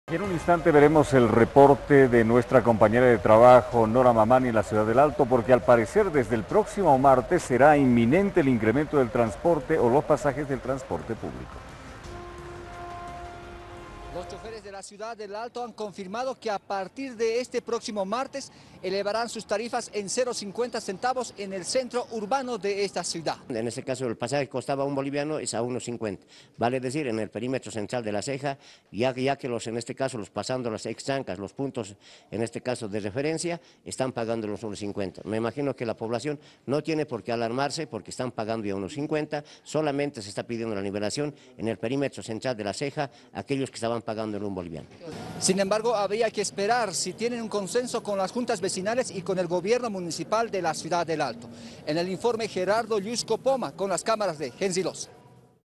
Choferes confirman incremento de pasajes en El Alto -Jueves 5 de mayo de 2016
(Abya Yala Digital).-  Desde el próximo martes 9 de mayo, se incrementará la tarifa del transporte público en la ciudad de El Alto. El incremento será de 0,50 centavos. (video)